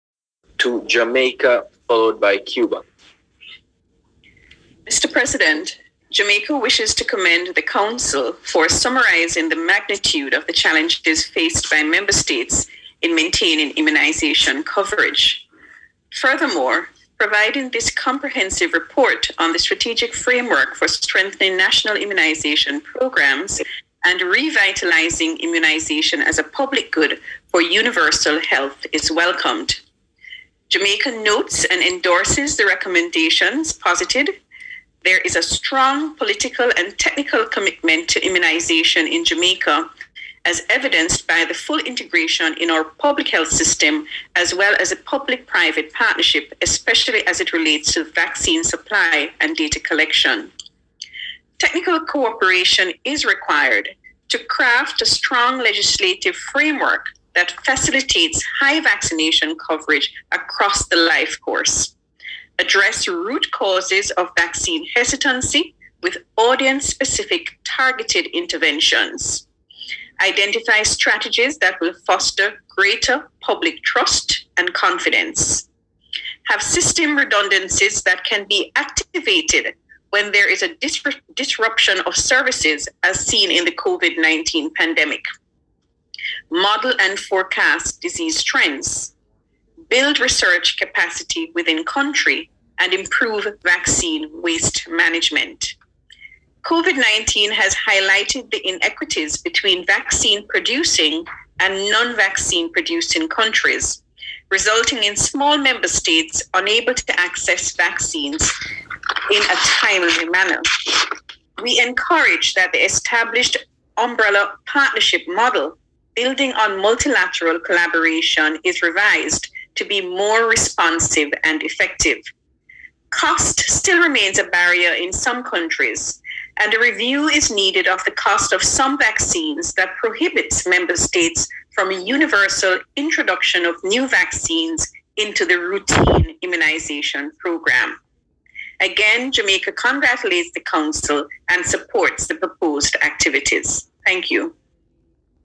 59th PAHO Directing Council Meeting
Jamaica's CMO notes position on technical cooperation and immunisation at today's sitting of the 59th Directing Council meeting in Washington DC, chaired Minister